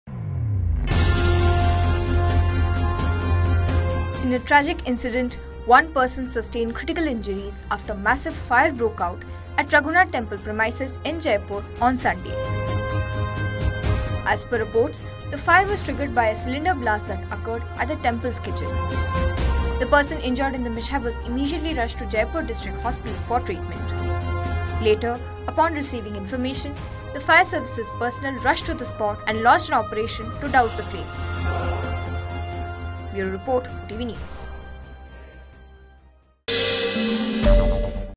WATCH | Massive Fire After Cylinder Blast In Odisha Temple Leaves One Person Critically Injured
In a tragic incident, one person sustained critical injuries after a massive fire broke out at Raghunath Temple premises in Jeypore (Koraput) on Sunday (March 20).